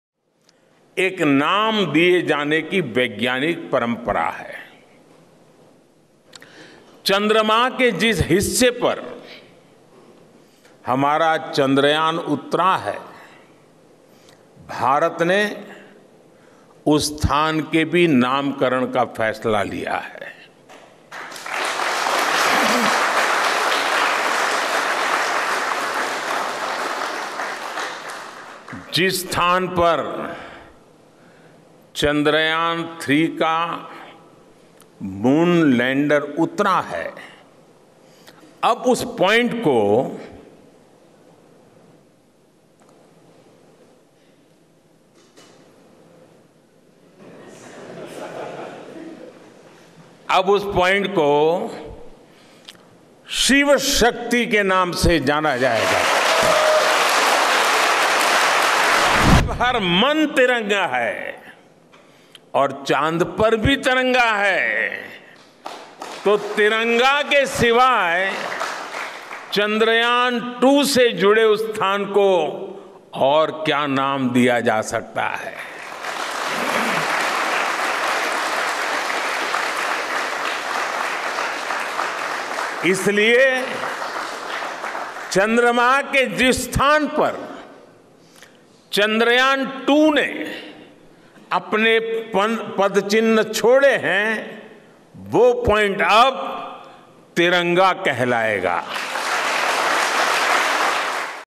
Chandrayaan 3: चांद पर जहां उतरा चंद्रयान-3 का लैंडर, उस जगह का नाम रखा गया | Shivshakti
चांद के दक्षिणी ध्रुवीय क्षेत्र में सफल लैंडिंग करने वाला पहला देश बना भारत. चांद के दक्षिणी ध्रुव के बारे में माना जाता है कि यहां बने गड्ढे अंधेरे में रहते हैं और उनमें पानी होने की उम्मीद है. चंद्रयान-1 ने चांद पर वॉटर मॉलीक्यूल्स की खोज में अहम भूमिका निभाई थी, जिनको लेकर और जानकारी जुटाने की भी जरूरत है.